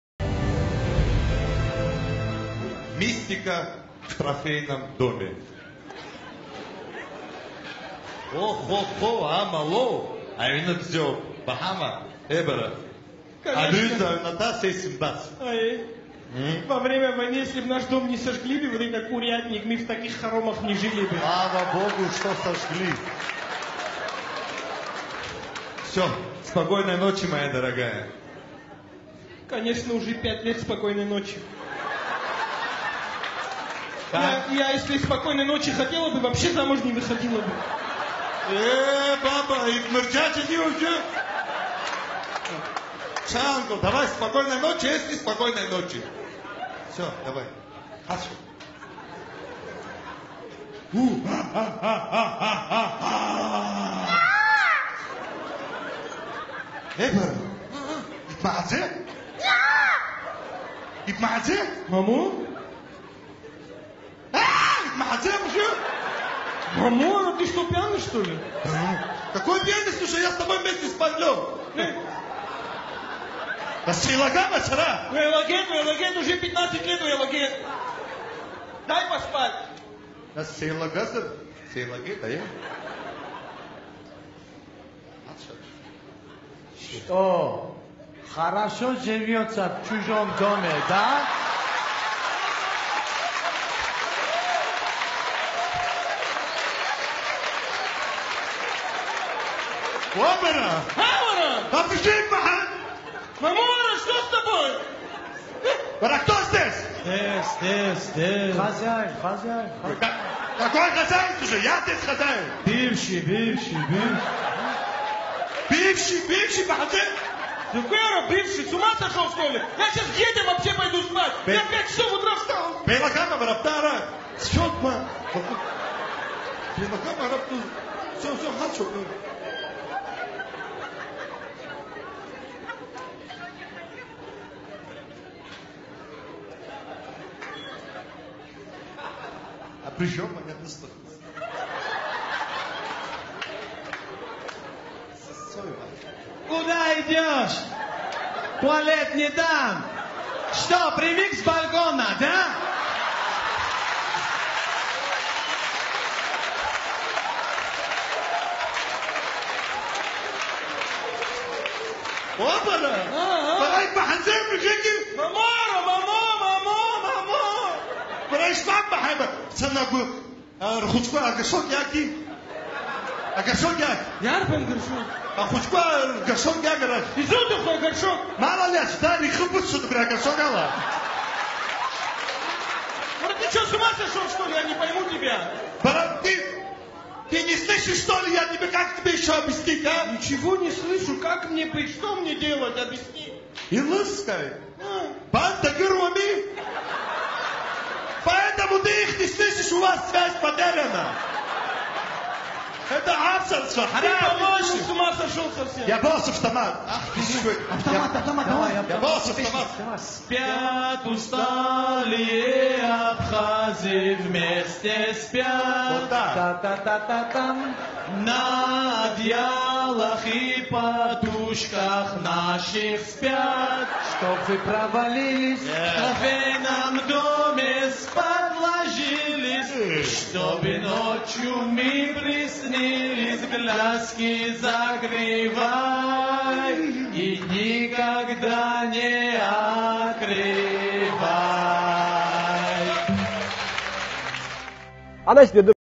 мистика в трофейном доме
абхазу  живущему в трофейном грузинском доме, ночью являются его прежние убитые хозяева.